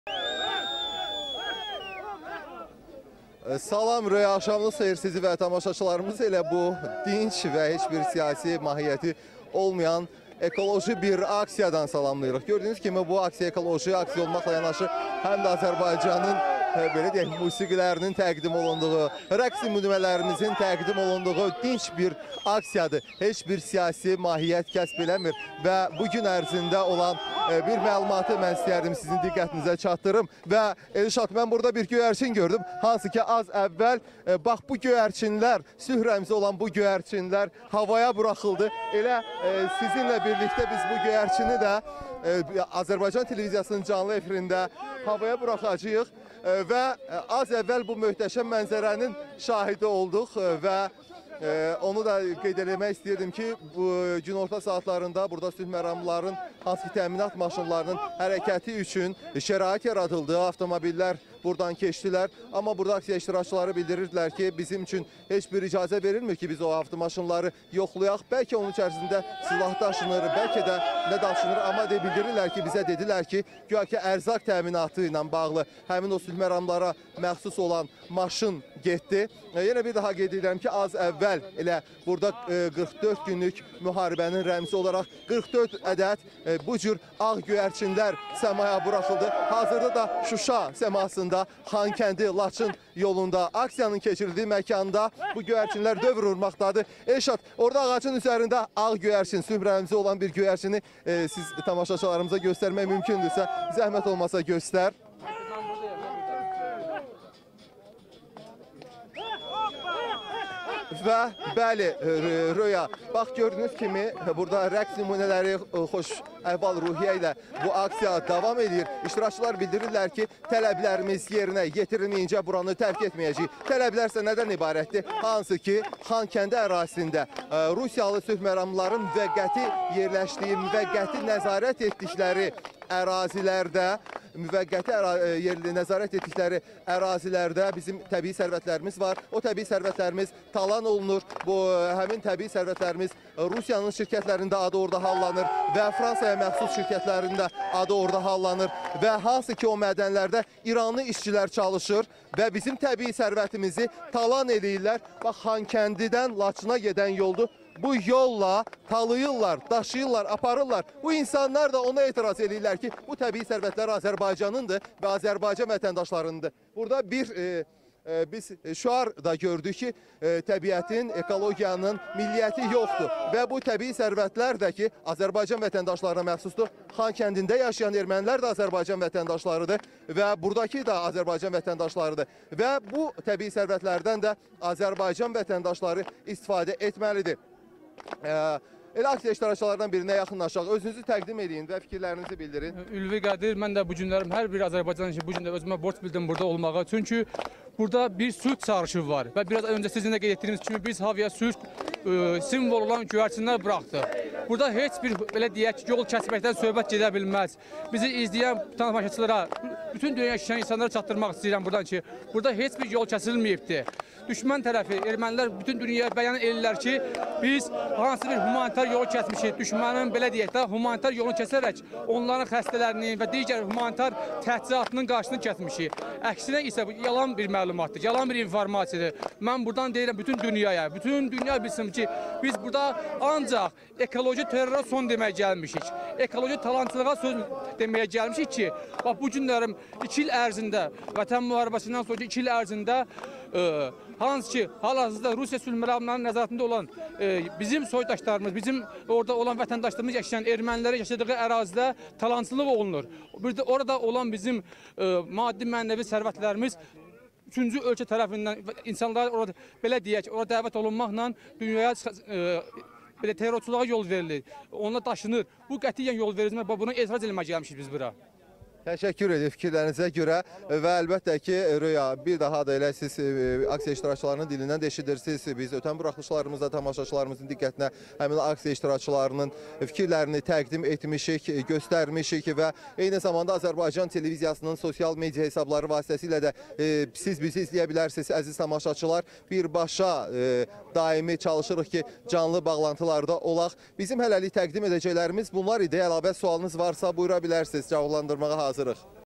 Xankəndi yolunda etiraz aksiyası davam edir: havaya 44 ədəd göyərçin buraxıldı – BİRBAŞA BAĞLANTI
ŞUŞADA AZƏRBAYCANLILARIN ETİRAZ AKSİYASI DAVAM EDİR